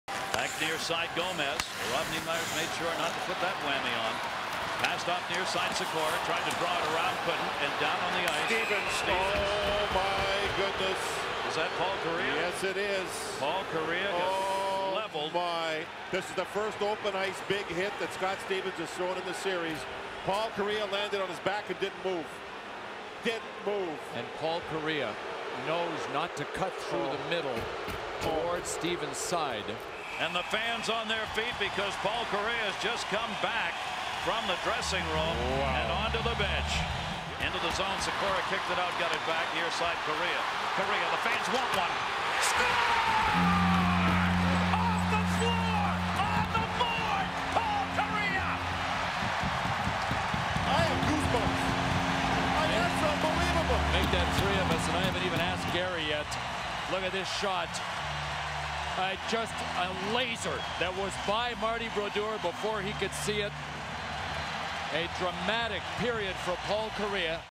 Down But Not Out Paul Kariya '03
One of the most dramatic moments ever in the Stanley Cup Finals. With his team down 3 to 2 in game 6 of the Stanley Cup Final, Ducks captain, Paul Kariya cuts into the middle of the ice at the pond in Anaheim. As Kariya dumps the puck into the New Jersey zone, Devils captain Scott Stevens pinches in and delivers a signature devastating hit. Kariya's head is rocked into the hard ice and his visor is knocked into the front of his face. After lying motionless for several seconds, Kariya "came back to life" with a cough that produced a cloud of condensation on his visor. Minutes later, Kariya dramatically returns to the Anaheim bench and proceeds to score the game winning goal with a rocket of a slap shot past Devils goaltender Martin Broduer.